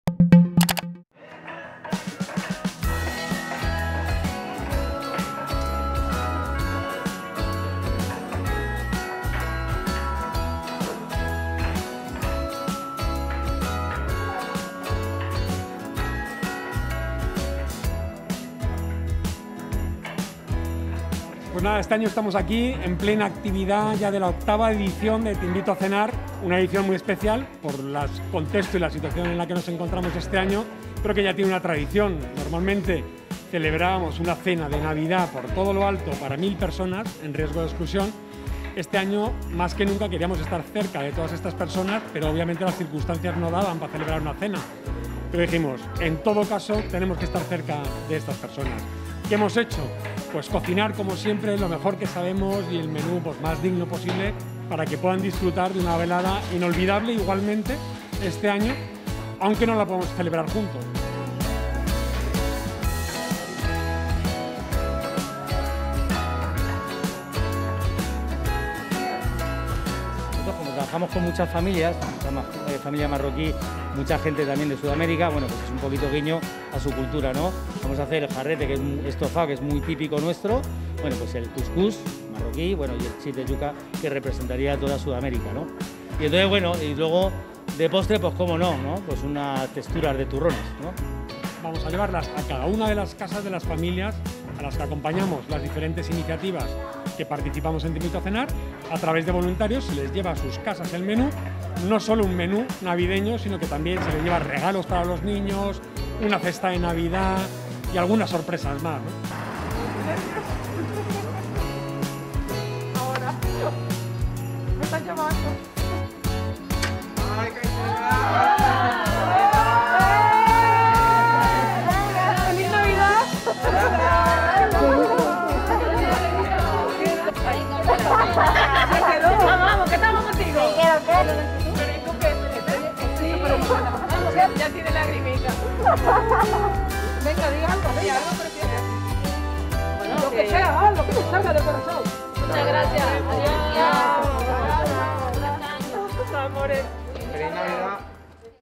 'Te invito a cenar': menús navideños a domicilio para gente sin recursos
Madrid, 20 dic (EFE).- (Imagen: Tato Pérez y Enrique del Viso) Este domingo tiene lugar la octava edición de "Te invito a cenar", una iniciativa que, en esta ocasión, llevará una cena de Navidad especial a los hogares de mil personas sin recursos, con la ayuda de grandes chefs.
Atendiendo a las disposiciones sanitarias, no se celebrará la multitudinaria cena de años anteriores. Sin embargo, "Te invito a cenar" sigue celebrando la Navidad de forma especial con los más vulnerables, los que más han sufrido los efectos de la crisis de la covid-19.
A los hogares de unas mil personas, trescientos voluntarios llevarán estos menús especiales, elaborados por cuarenta chefs y compuestos de aperitivos, primero, segundo plato y postre.
Además, habrá una cesta con alimentos y productos básicos y en los hogares donde haya niños, se enviarán juguetes, anticipando la llegada de los Reyes Magos.
Los chefs implicados en esta iniciativa son, entre otros, María Jiménez, Juan Pozuelo, Rubén Cordero, José Luis Inarejos, Chema de Isidro, Pepe Gorines, Santiago Abad y Nico Ríos, Eduardo Casquero, David Rubio o Anita Be Cool.
El Mirador de Cuatro Vientos ha sido el lugar donde los chefs han ultimado los platos y los voluntarios han preparado las cestas para salir a los hogares.